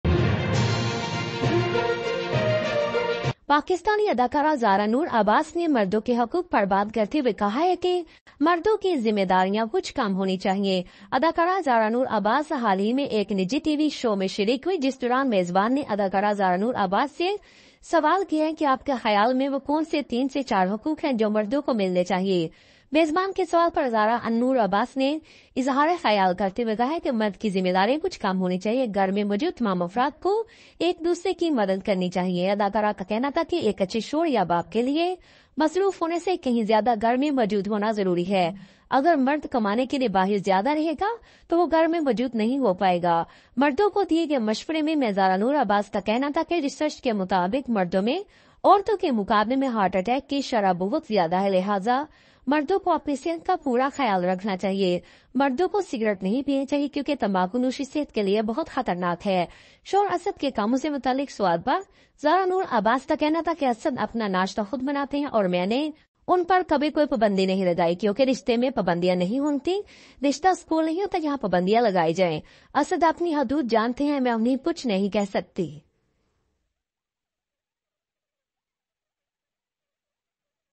PAKISTANI ACTRESS ZARA NOOR ABBAS VIRAL INTERVIEW |N3HD | SUFIA MALIK | زارا نور مردوں کی حمایت میں
زارا نور عباس مردوں کی حمایت میں سامنے آگئیں,